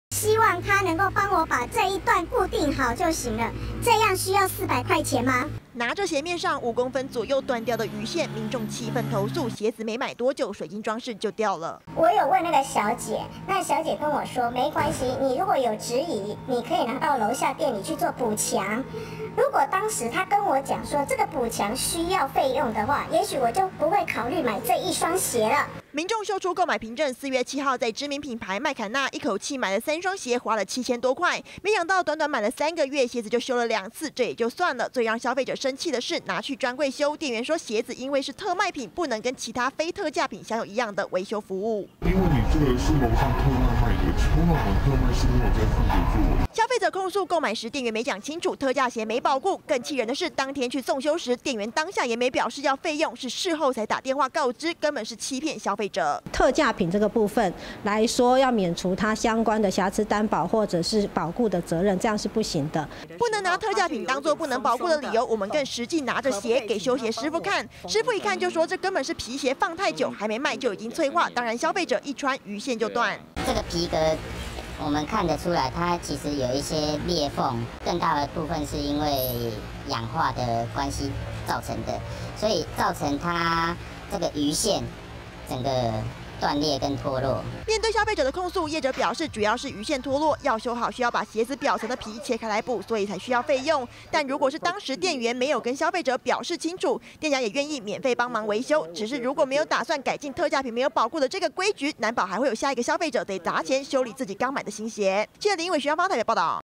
20180725中天新聞 3個月新鞋補線要400元 店家：特價鞋不保固
特價品的保固服務，跟一般商品居然不同，就有民眾投訴，一雙才買3個月的新鞋，就發生鞋面裝飾品脫落的狀況，沒想到找店員維修，居然縫個魚線，就要價400元，讓民眾氣憤說，買的時候店員沒講清楚維修的服務條款，事後才說特賣品不保固，感覺被騙了，對此店家回應，如果因為店員工溝通問題，這次會提供免費維修。